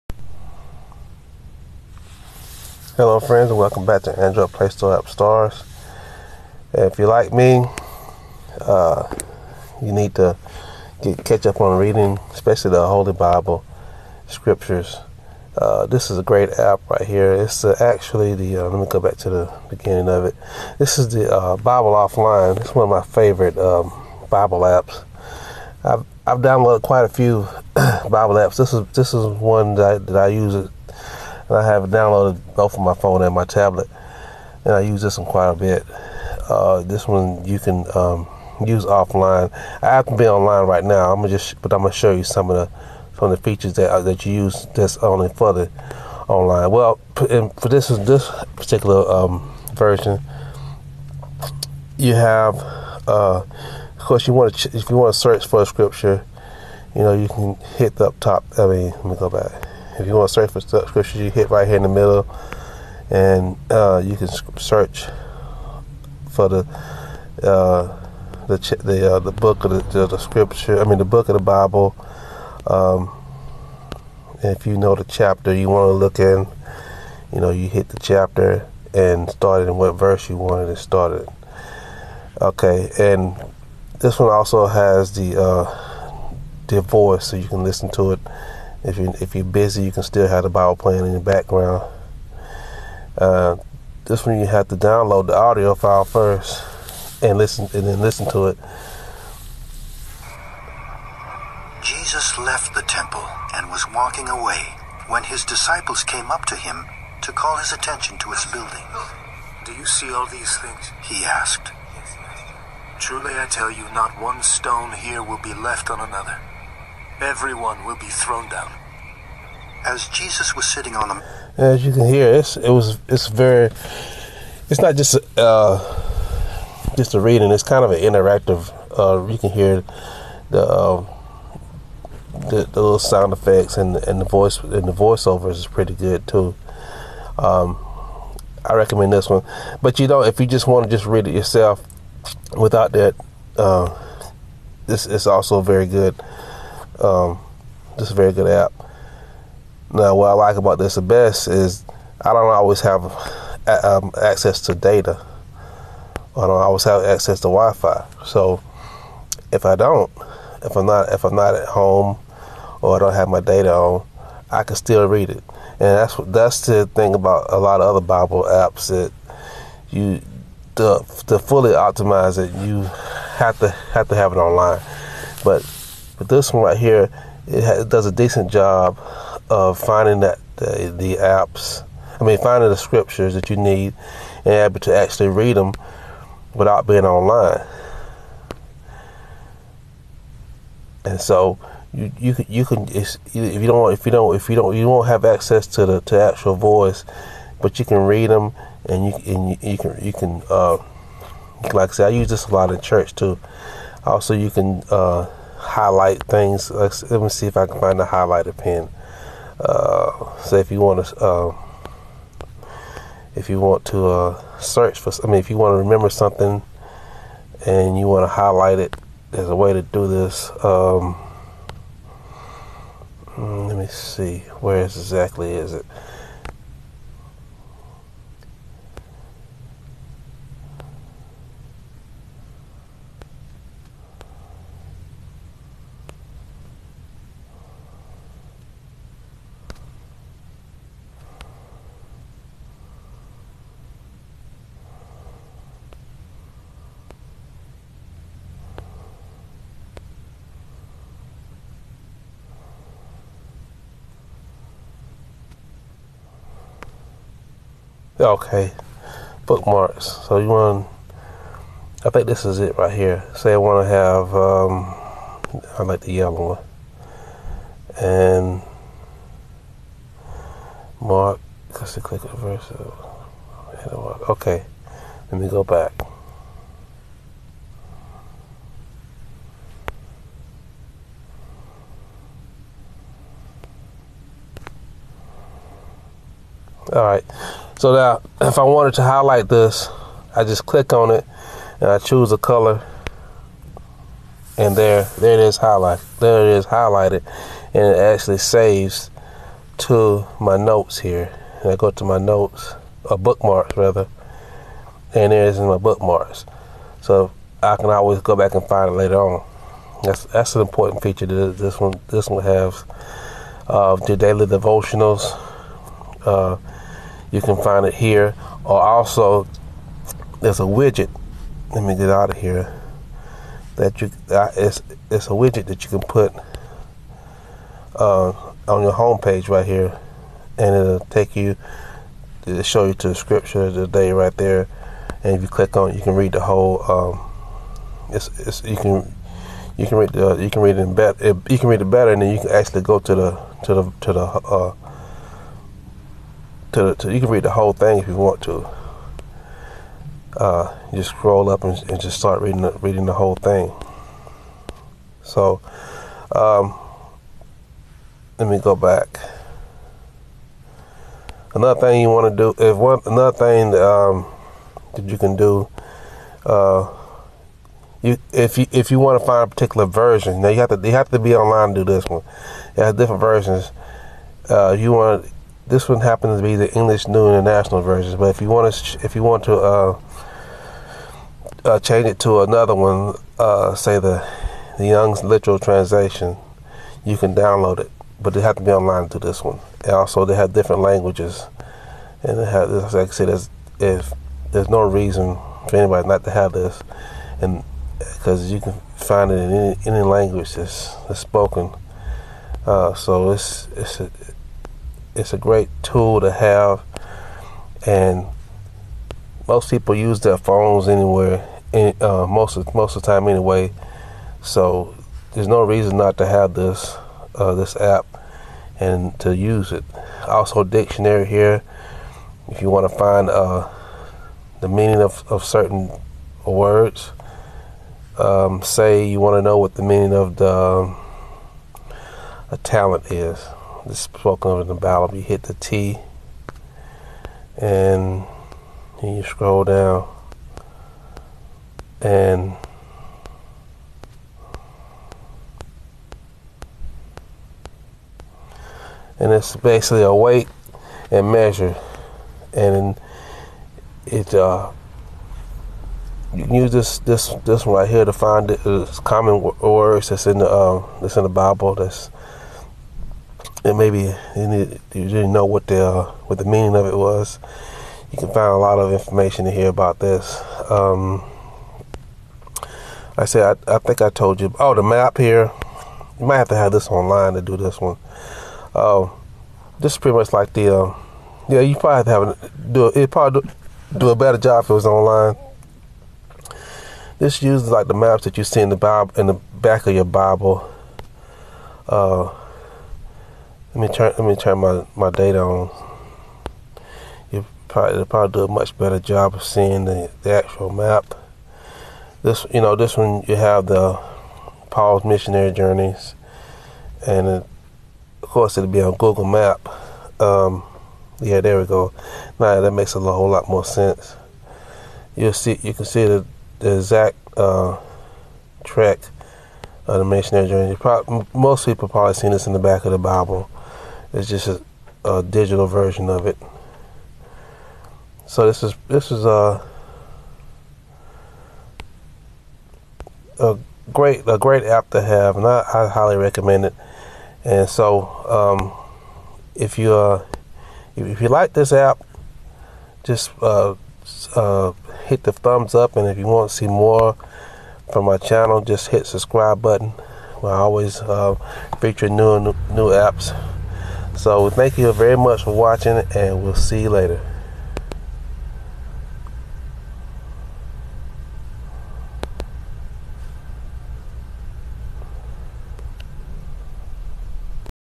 Best Off Line Bible App In Google Play Store Tutorial And Review
With all the bible app available there is only one I have found that suits my needs both online and off with it features.